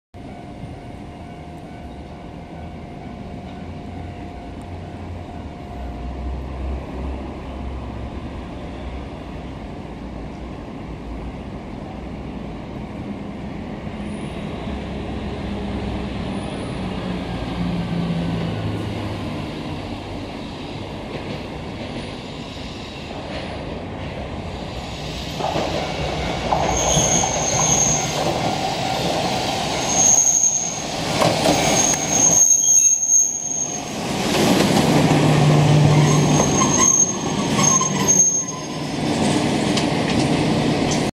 阪堺電気軌道上町線 松虫停留所上りホームにモ701形が到着
2020年9月5日撮影

阪堺電気軌道上町線　松虫停留所上りホームにモ701形が到着します。